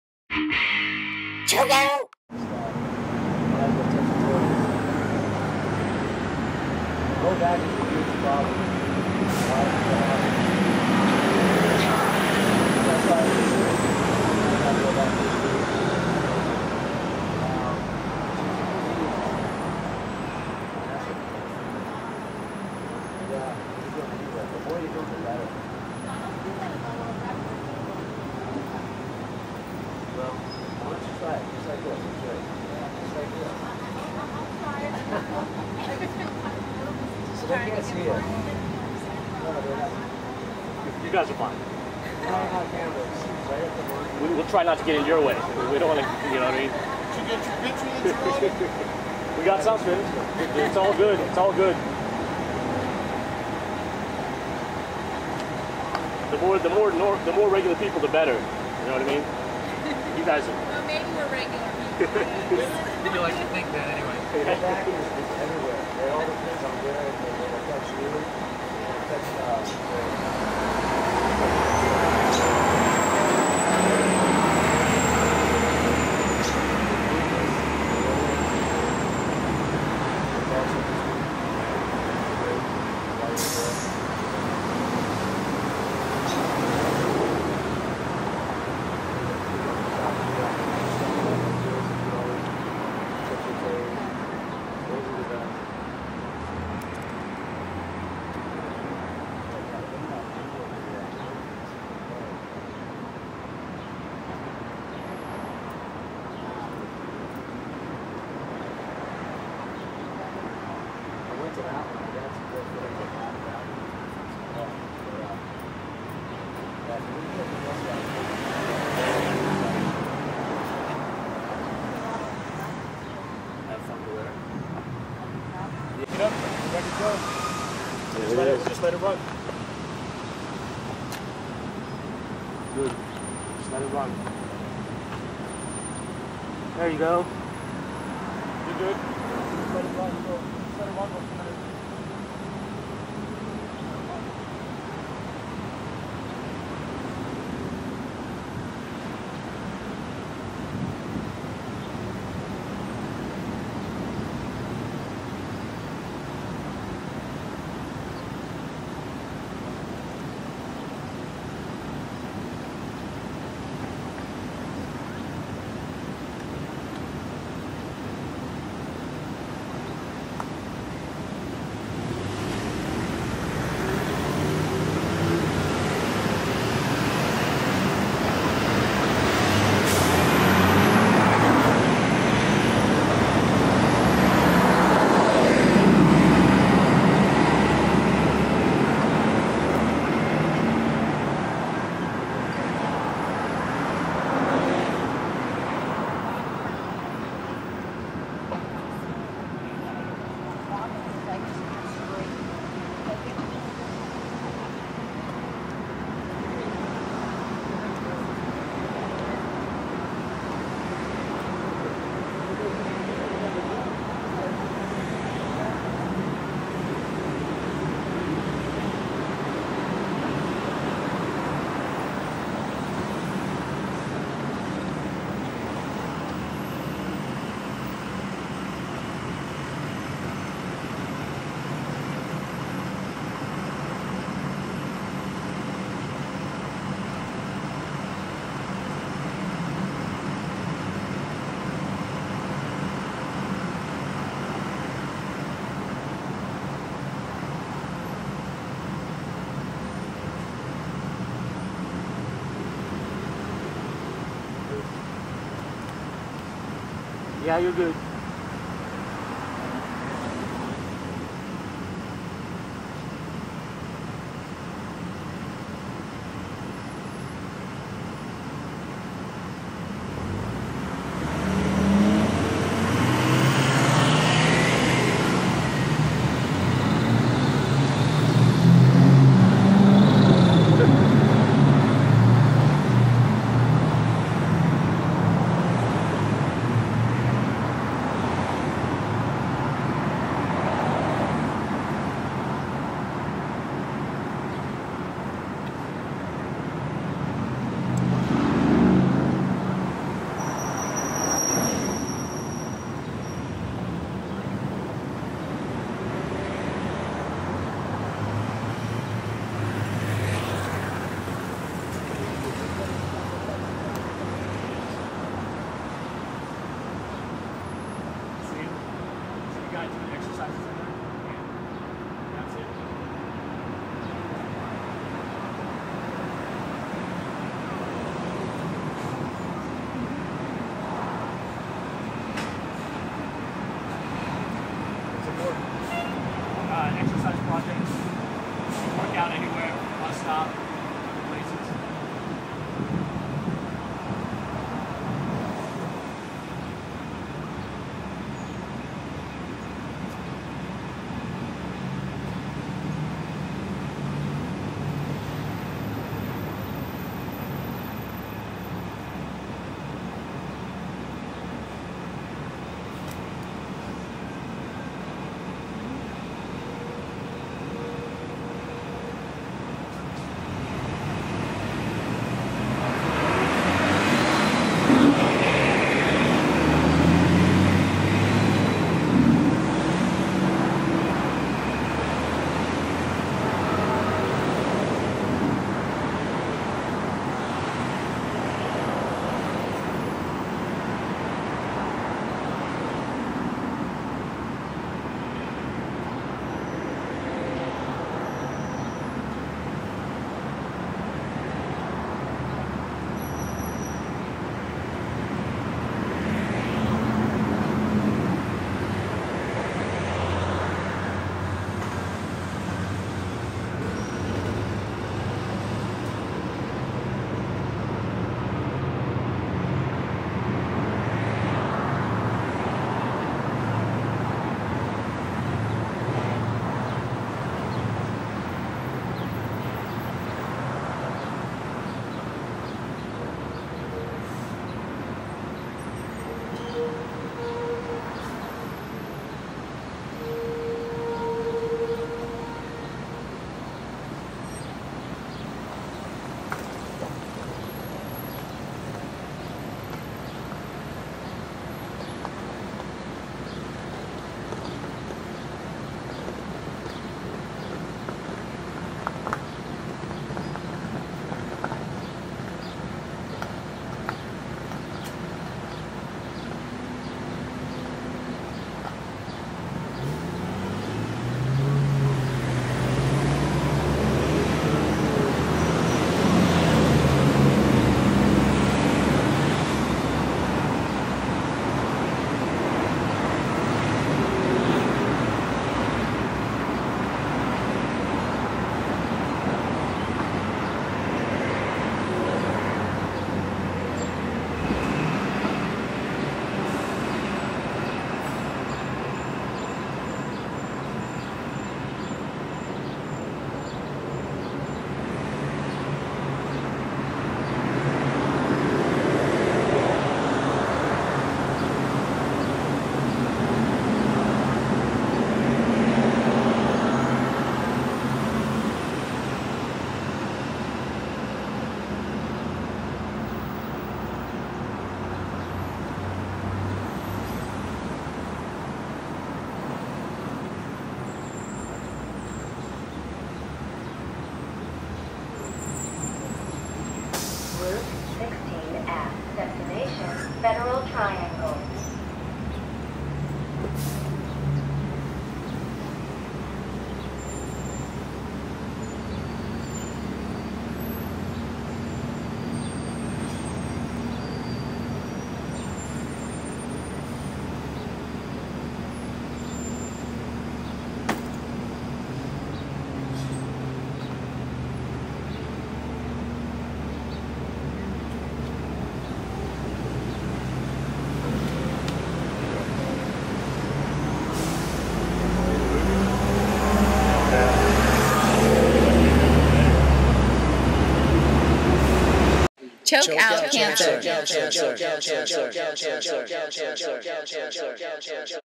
Bus Stop WorkOuT Washington Monument 4
Millions of people ride the Bus everyday around the world.  ChokeOuT Cancer shares some great videos on workouts that anybody can do while waiting at the Bus Stop.  Most people wait a minimum of 15 minutes for the Bus to get to and from so why not use that time to get a little workout in.  ChokeOuT Cancer is about being active and pro-active.  I've traveled the world finding the best bus stop locations with the best backdrops to share the ChokeOuT Cancer Bus Stop WorkOuTs.  Look through the glass and you can see the Washington Monument in the background